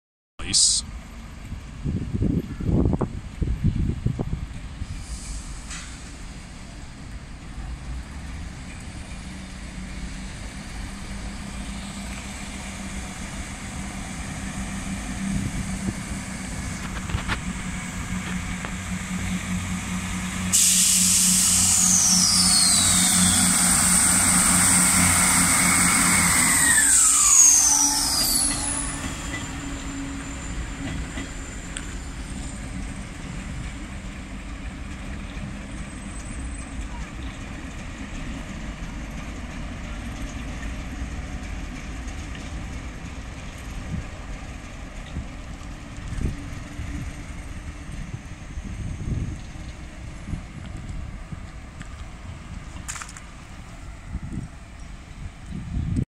Class 08 Going Through The Sidings And Southwestern Leaving Eastleigh For Romsey
2nd Video Of Eastleigh Train Station